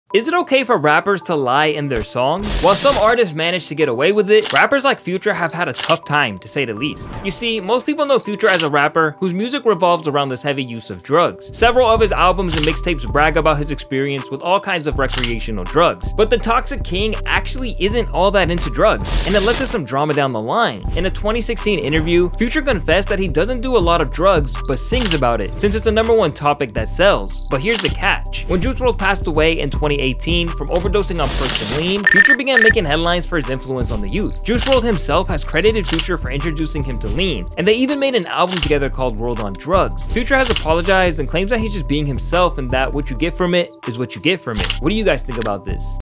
Rappers WHO LIED in their songs...
Rappers lying in their music happens more often than telling the truth. However, what happens when those same songs inspire the youth to dabble take action on those lies cuz they like your music? This was the case for Juicewrld and Future back in 2019, after juicewrld unfortunately passed away after overdosing on l**n and p**cs. He once credited Future for getting him into drugs, to which Future has apologized... thoughts?